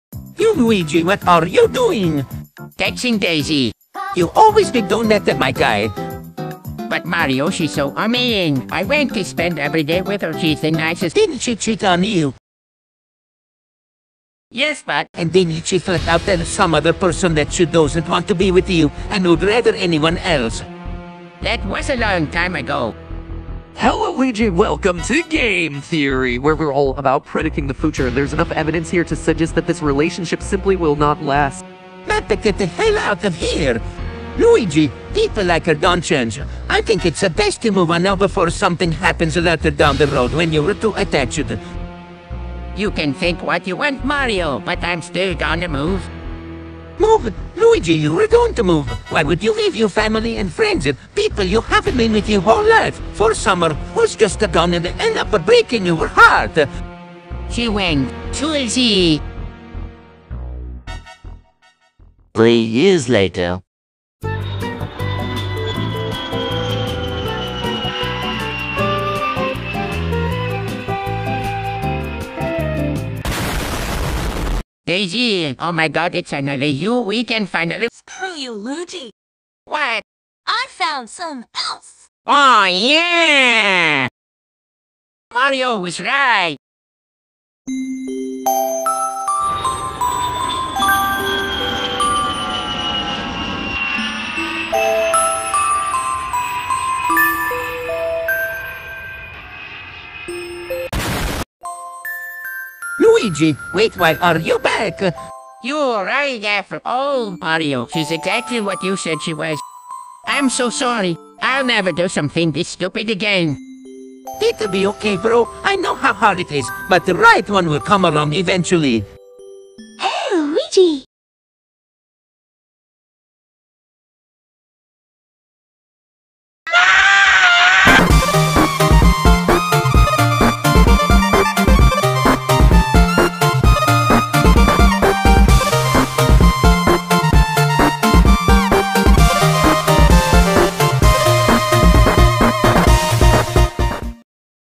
Luigi's Mistake
I normally never make these kind of videos, but I had a creative spark. I'm sure you've seen those Mario TTS videos on YouTube before (likely in your recommended). I always thought they were hilarious, and I always wanted to do one myself. Now I have and it took way longer than it should've to make.